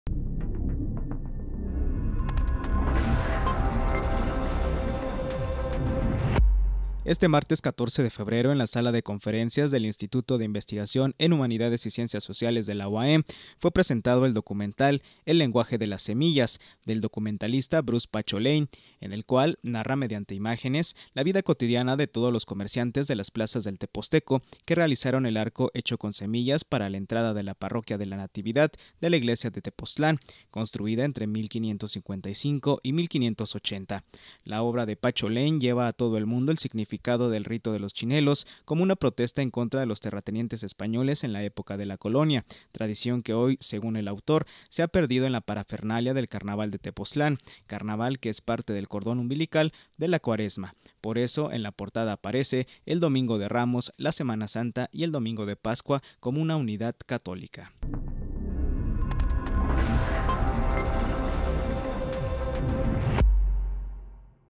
Proyectan documental El lenguaje de las semillas
Ciudad Universitaria a 14 de febrero de 2017.- Este martes 14 de febrero en la sala de conferencias del Instituto de Investigación en Humanidades y Ciencias Sociales de la UAEM, fue presentado el documental "El lenguaje de las semillas" del documentalista Bruce "Pacho" Lane, en el cual narra mediante imágenes, la vida cotidiana de todos los comerciantes de las plazas del Tepozteco que realizaron el arco hecho con semillas para la entrada de la Parroquia de la Natividad, de la Iglesia de Tepoztlán, construida entre 1555 y 1580.

La obra de Pacho Lane, lleva a todo el mundo el significado del rito de los chilenos como una protesta en contra de los terratenientes españoles en la época de la colonia, tradición que hoy según el autor se ha perdido en la parafernalia del carnaval de Tepoztlán, carnaval que es parte del cordón umbilical de la cuaresma, por eso en la portada aparece el "Domingo de Ramos", la "Semana Santa" y el "Domingo de Pascua" como una unidad católica.

Dirección de Comunicación Institucional

Coordinación de Información

Voz e información: Julio Román

Video: Humberto Ramírez

Fotografías: José Luis Arroyo

Edición: Juan Cadena